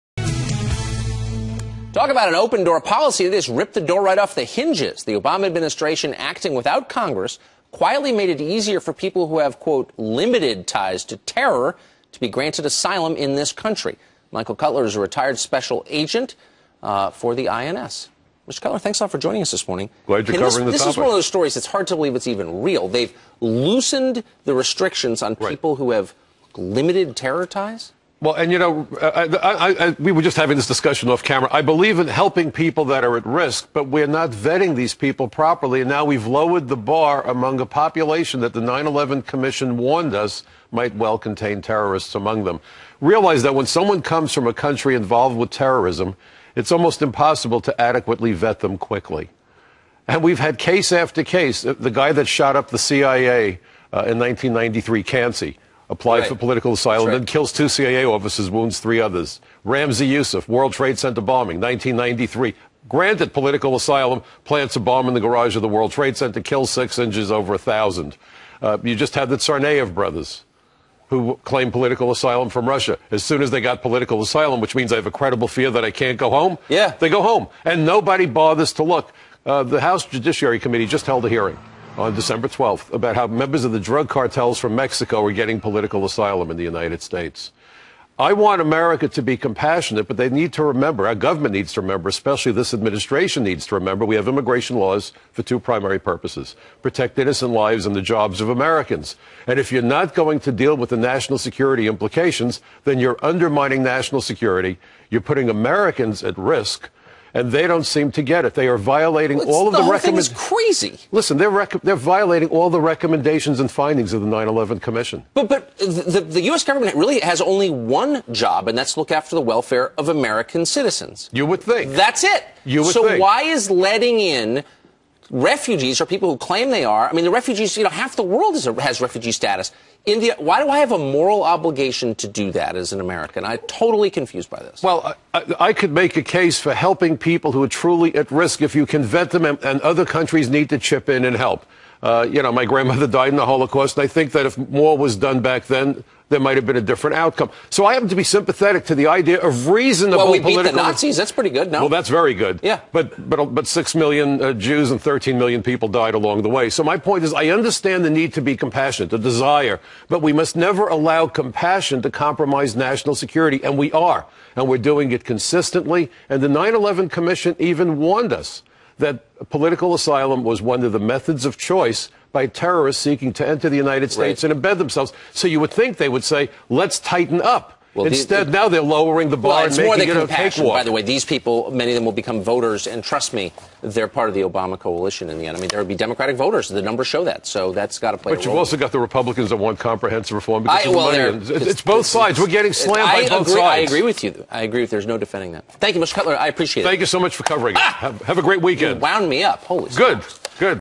New Immigration Exemptions Putting U.S. At Risk
'Loose' terror ties allowed for asylum.